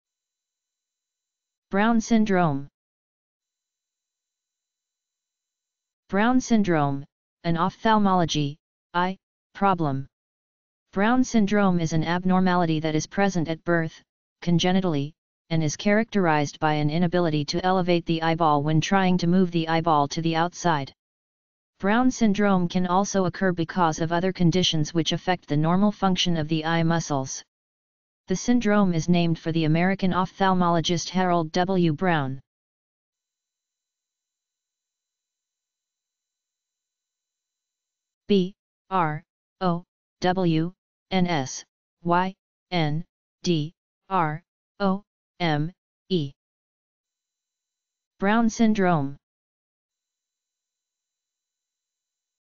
Brown syndrome - Medical Meaning
Brown syndrome

Brown syndrome: An ophthalmology (eye) problem. Brown syndrome is an abnormality that is present at birth (congenitally) and is characterized by an inability to elevate the eyeball when trying to move the eyeball to the outside. Brown syndrome can also occur because of other conditions which affect the normal function of the eye muscles. The syndrome is named for the American ophthalmologist Harold W. Brown.